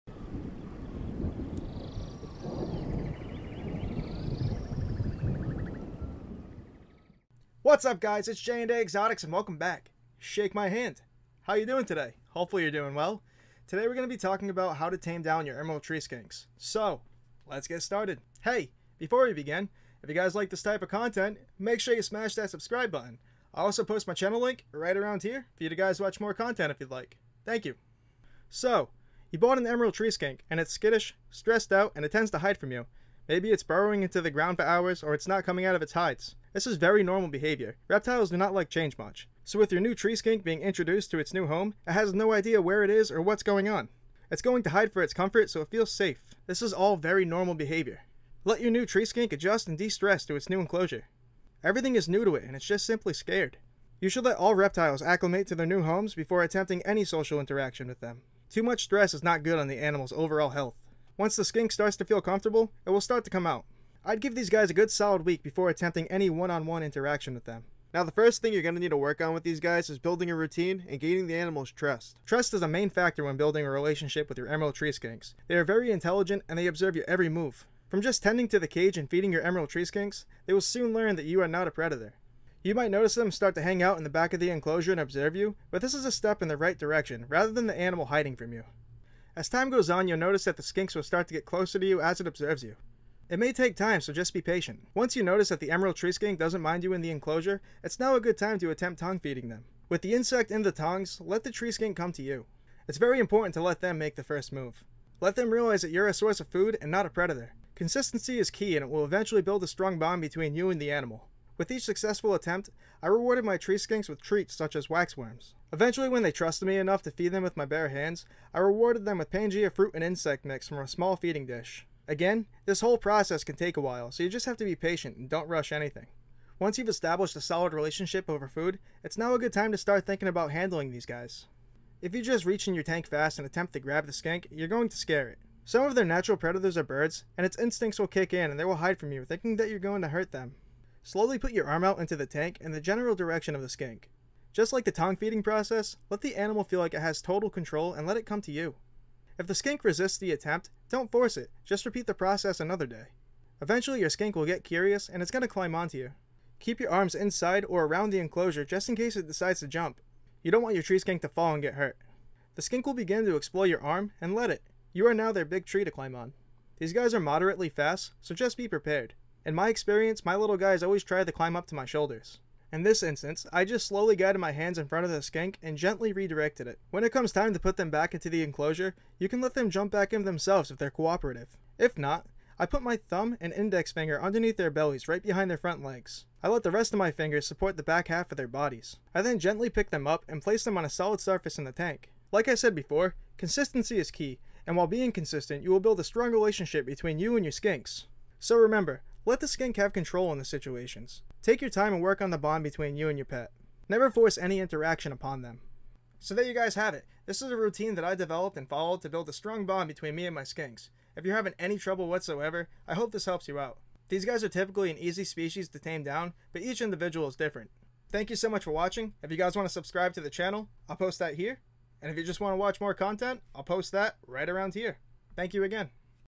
How To Tame Emerald Tree Skinks!
This is the step by step process process I made that helped me tame down my Emerald Tree Skinks and gain their trust! I've developed a great bond with my animals! If you are having any trouble taming down your tree skinks, I hope this helps you out! They are relatively easy to tame down but it just takes time!